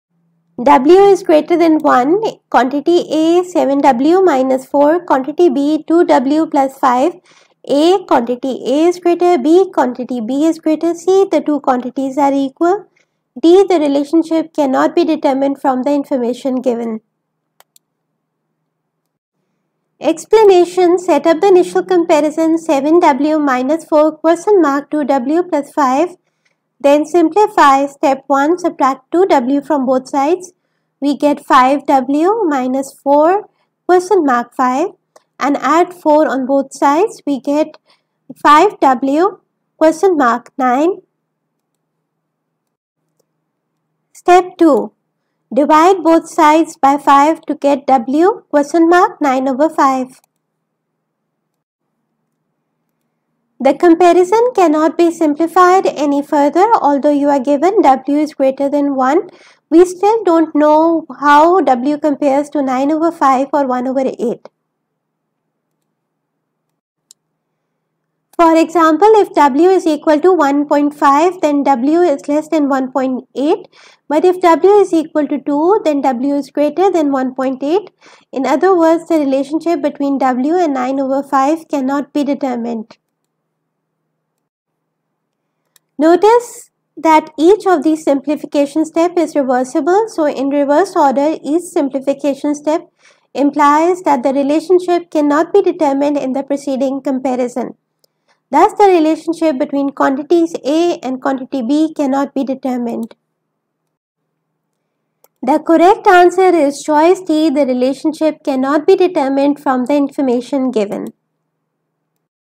9. Quantitative Reasoning by ETS Comparison of two algebraic expression
w is greater than 1
Quantity A: 7w −4
Quantity B: 2w + 5